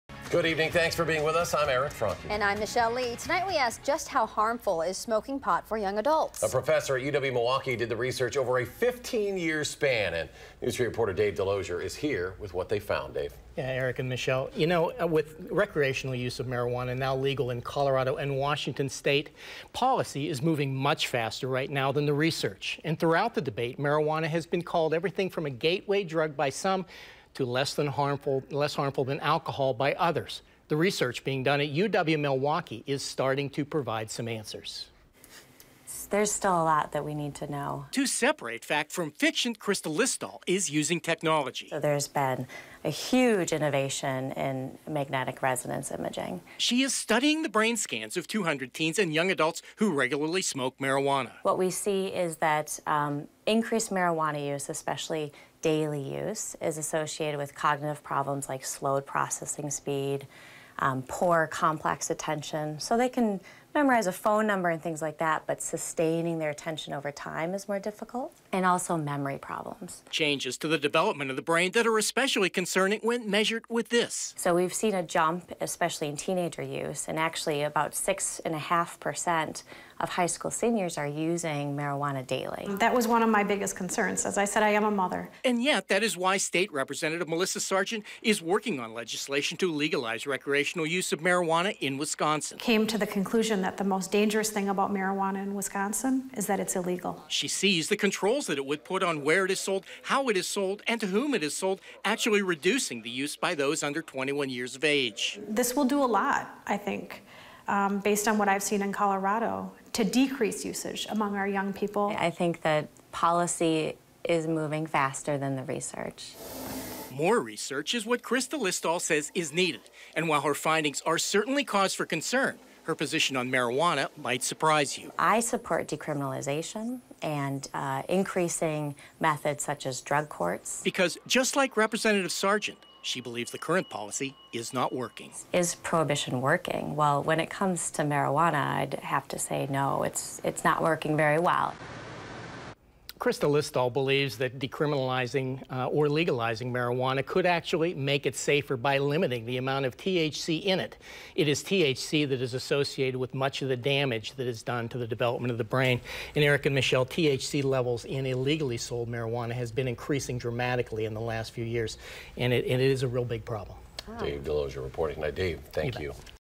Research shows negative effect marijuana has on development of teens
The images on the computer screen paint a picture of the impact marijuana has on the development of the brain of teens or young adults. Krista Lisdahl has been analyzing brain scans of 200 teens and young adults trying to determine the impact of smoking marijuana on a regular basis.